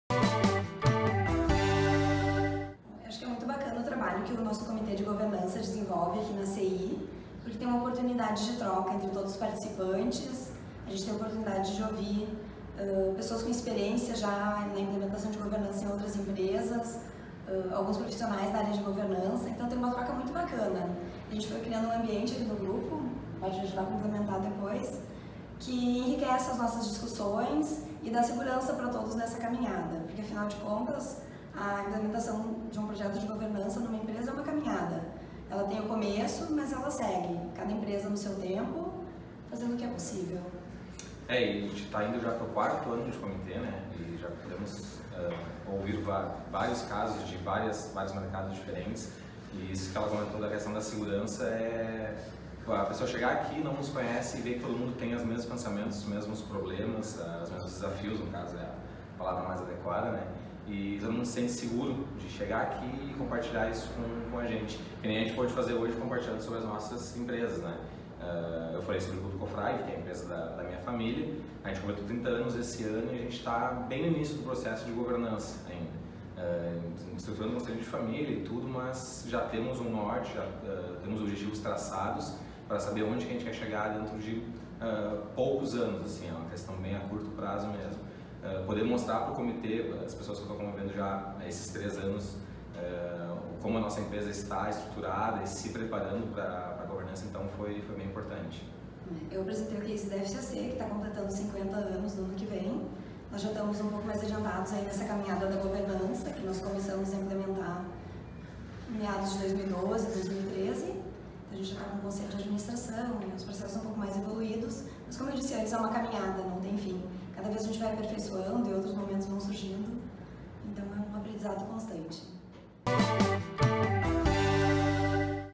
Cases dos grupos Cofrag e FCC são apresentados no Comitê de Governança Corporativa
Pensando na importância do desenvolvimento pessoal e profissional dos integrantes do Comitê de Governança Corporativa, na reunião de agosto e novembro deste ano o grupo promoveu a atividade de apresentação dos cases das empresas que integram o Comitê. Na primeira ocasião houve a exposição de três cases: André Kehl Schmitt, do Grupo Box Print, Deise Weber, da Calçados Piccadilly, e Rodrigo Koetz de Castro, da Teevo.
Nesta segunda oportunidade, Christian Haag, do Grupo Cofrag, e Letícia Reichert Vieira, do Grupo FCC, apresentaram um panorama de suas empresas, principalmente no que tange a estruturação da Governança Corporativa.